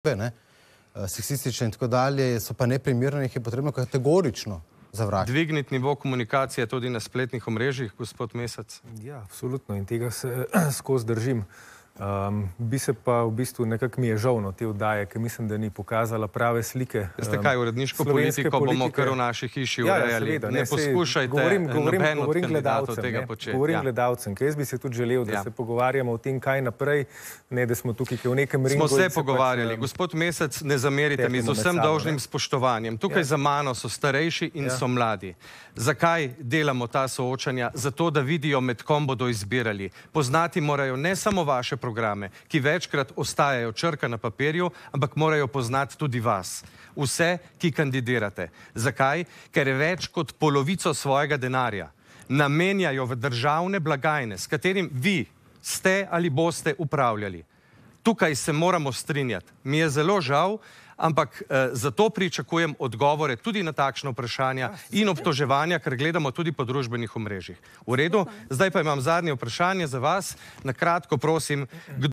Odločitev 2022: Politiki voditelju Janiju Muhiču dvignili pokrov
Celotna slovenska politika postaja peskovnik ekstremno nevzgojenih mulcev, ki se brez zadržkov obmetavajo z žaljivkami in vulgarnimi izrazi za mesto v ograjenem igrišču.